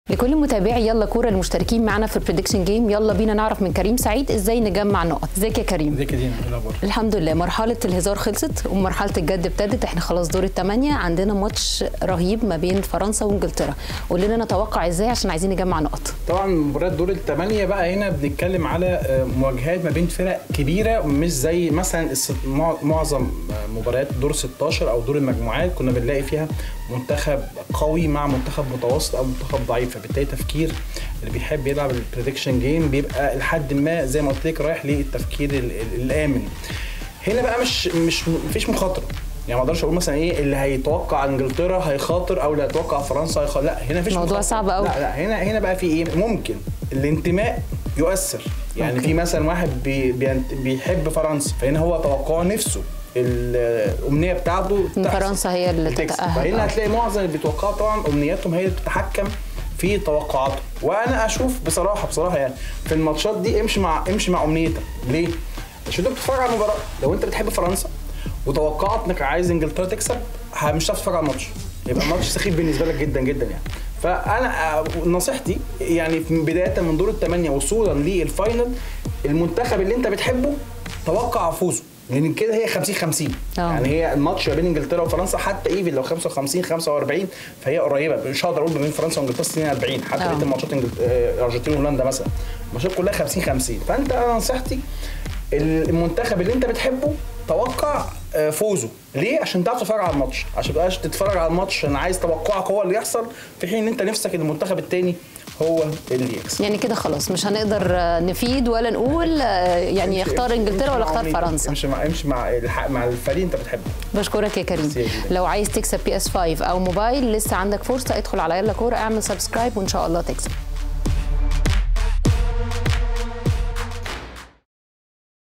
توقعات_المونديال.. إنجلترا ولا فرنسا؟ امشي ورا فريقك.. نصائح يلاكورة للفوز بـ بلاي ستيشن 5
توقعات_المونديال.. إنجلترا ولا فرنسا؟ امشي ورا الفريق اللي بتشجعه.. نصائح يلاكورة للفوز بـ بلاي ستيشن 5